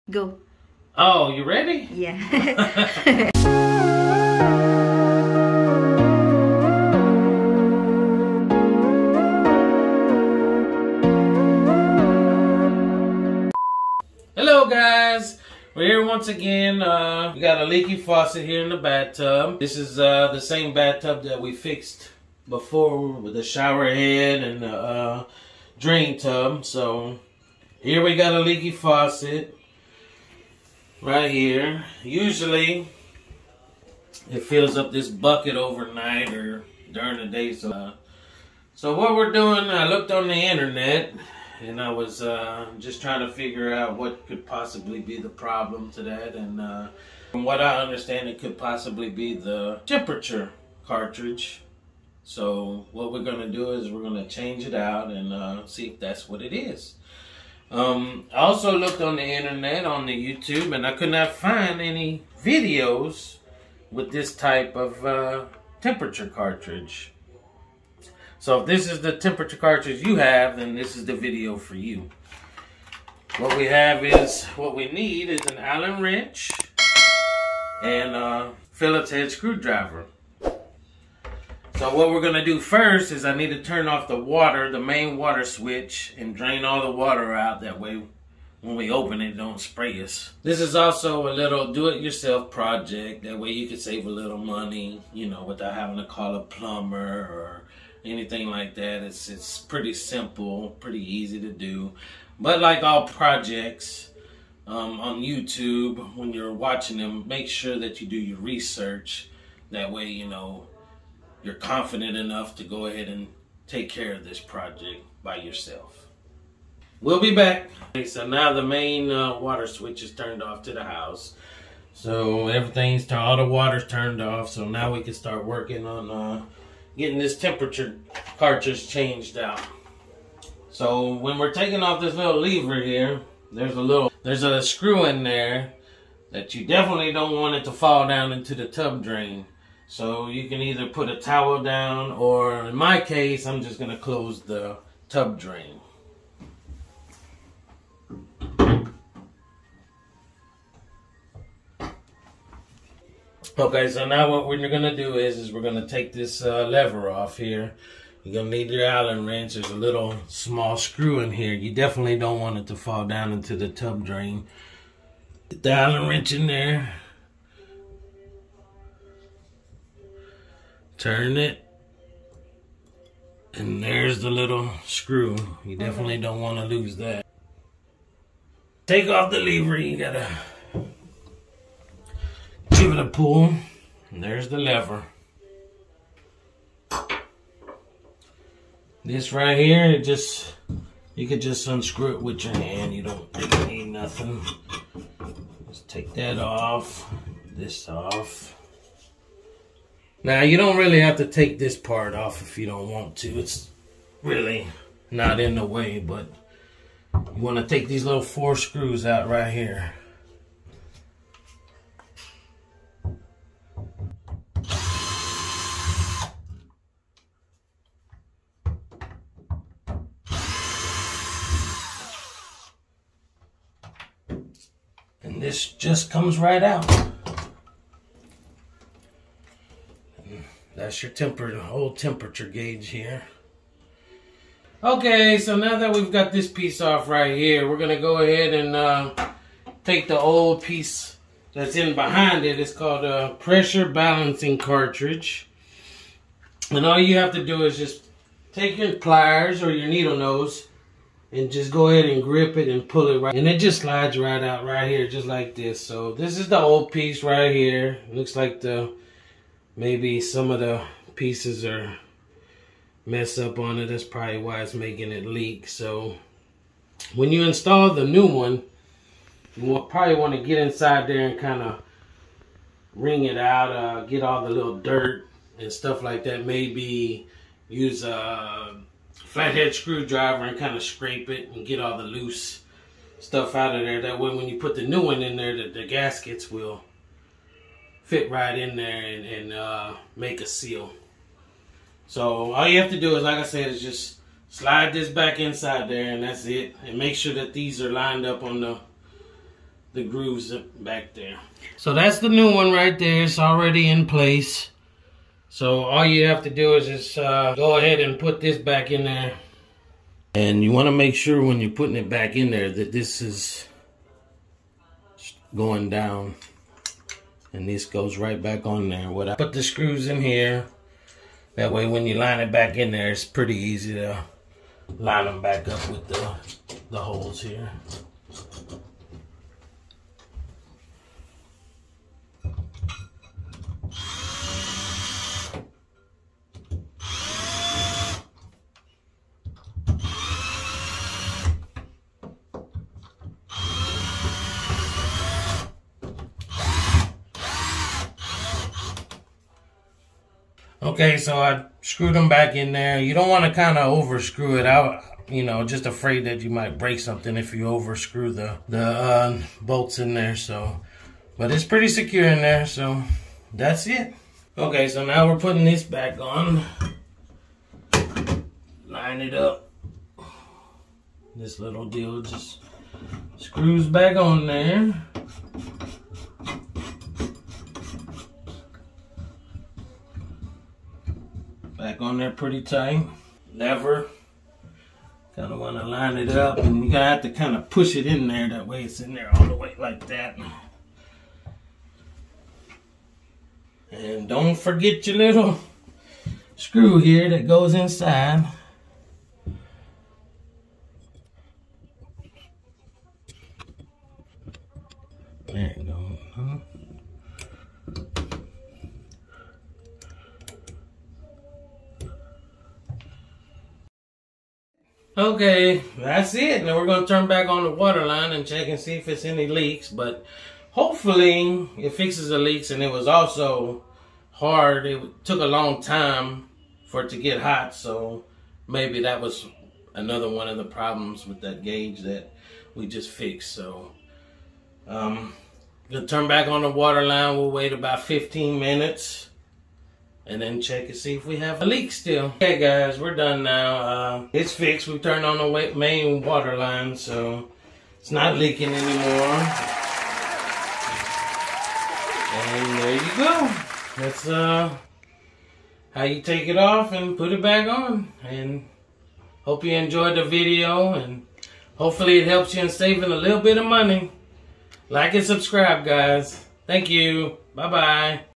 Fixing leaking bathtub faucet| Changing temperature cartridge & pressure gauge| Easy step
In this video, we will show you a simple diy quick fix of a leaking bathtub faucet. Save alittle money but of course do your research first before starting this project. Hope you will like it!